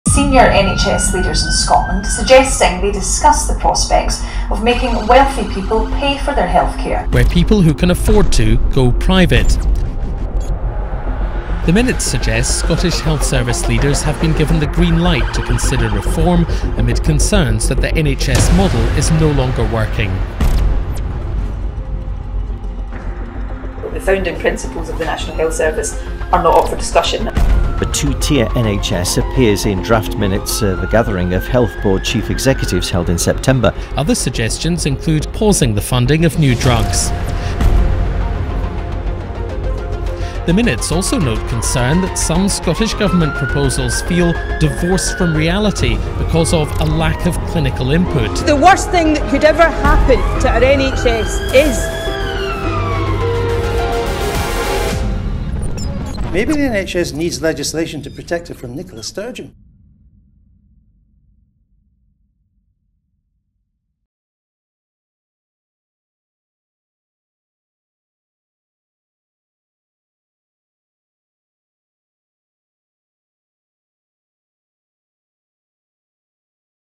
Protect the NHS (from Nicola Sturgeon)
The privatisation of our NHS is being actively discussed under @theSNP
As predicted, maybe our NHS needs to be protected from